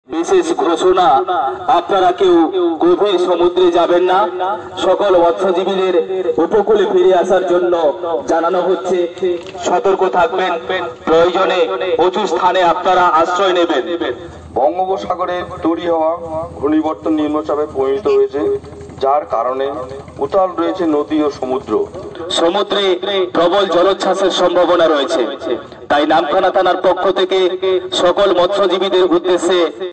প্রাকৃতিক দুর্যোগের পূর্বাভাসে Kakdwip এ Miking! বাড়তি সতর্কতা প্রশাসনের ।
n18oc_south-bengal । n18_shorts